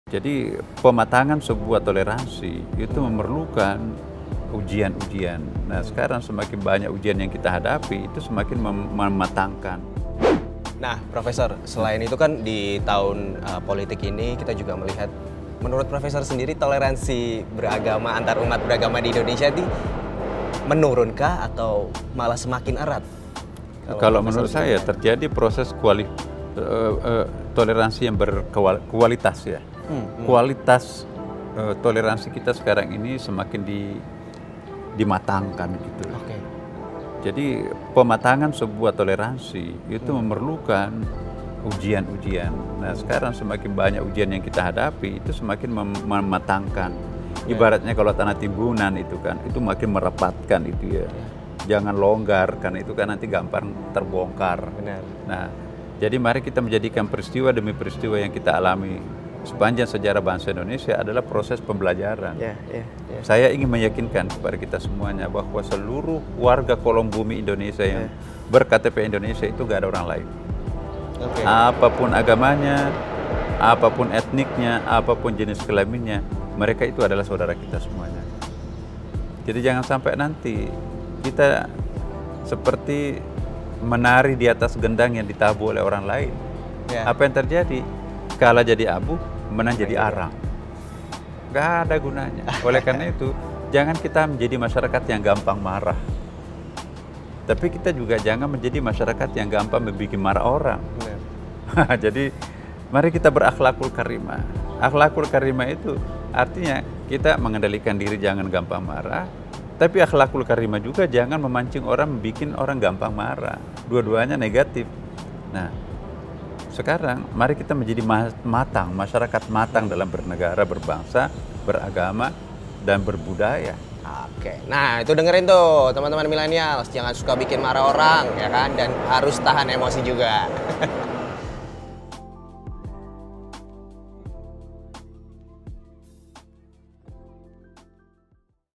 Toleransi Di Indonesia Semakin Berkualitas
Imam Besar Masjid Istiqlal Jakarta Prof. DR. K.H. Nasaruddin Umar,  menjelaskan mengenai toleransi yang ada di Indonesia. Beliau menjelaskan bahwa toleransi di Indonesia akan matang. Proses pematangan ini memerlukan ujian-ujian seperti sekarang ini, semakin banyak ujian maka toleransi akan semakin matang.

Baca selengkapnya di sini:
?utm_source=youtube&utm_medium=youtube&utm_campaign=youtube